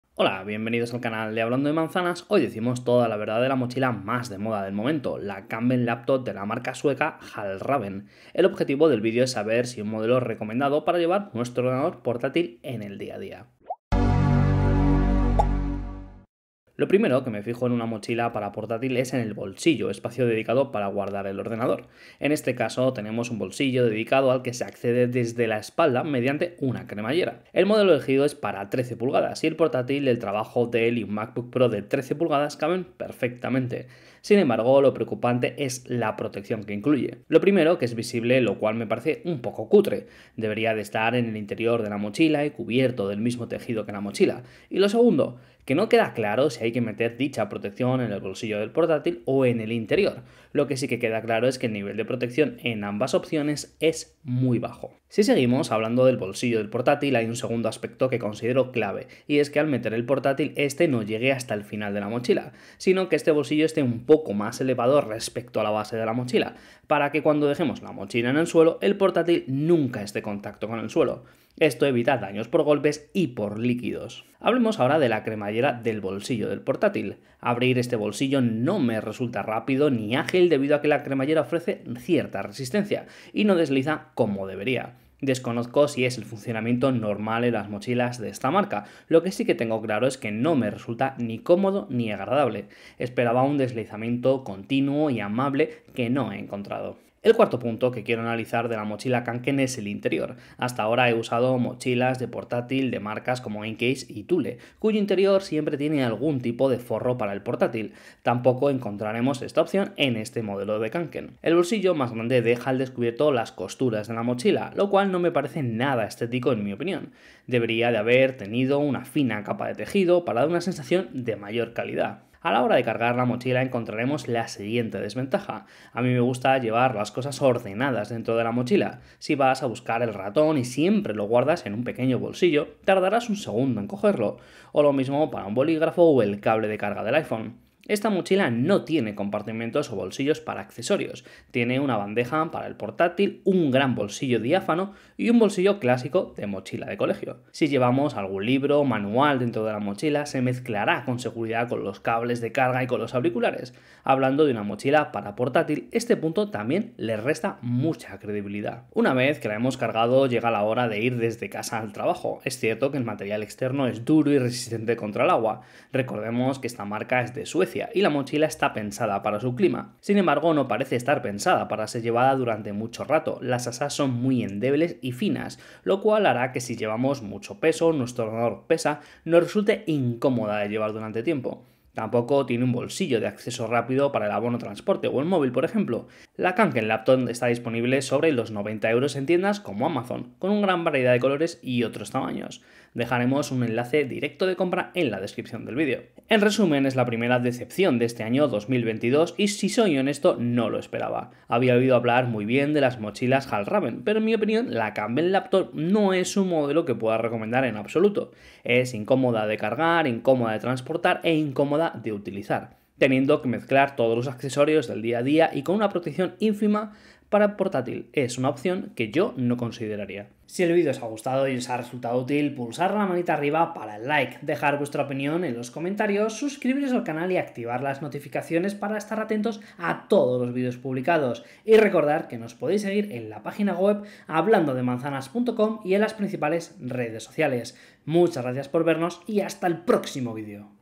Mochila Fjällräven Kanken Laptop · Análisis y Opinión
Fjällräven Kanken Laptop

Hoy decimos toda la verdad de la mochila más de moda del momento, la Kanven Laptop de la marca Sueca Fjällräven, el objetivo del video es saber si es un modelo recomendado para llevar nuestro ordenador portátil.

iPhone 12 Pro
Trípode Gorillapod
Micrófono Rode Mini
iPad Air 4 generación
Lumix GH5
Elgato Key Light